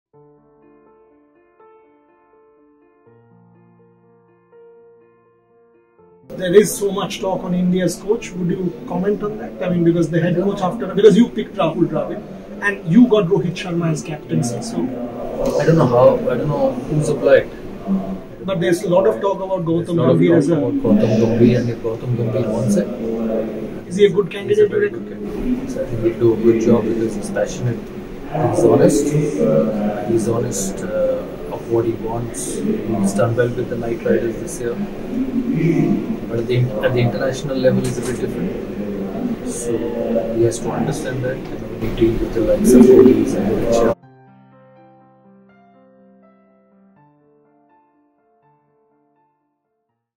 BWB Promo || Sourav Ganguly Believes that Gautam Gambhir is Perfect for India’s Head Coach role
🚨EXCLUSIVE

"He is passionate, he is honest...he is a very good candidate." Sourav Ganguly on Gautam Gambhir as a potential candidate for India's head coach position after Rahul Dravid.